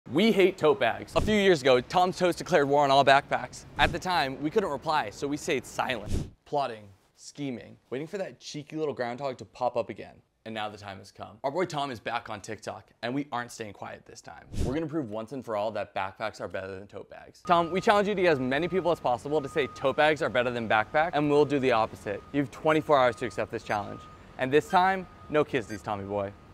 Declaring WAR on all TOTE BAGS!
Let this be our declaration of war on every unorganized, flimsy, and crappy tote bag!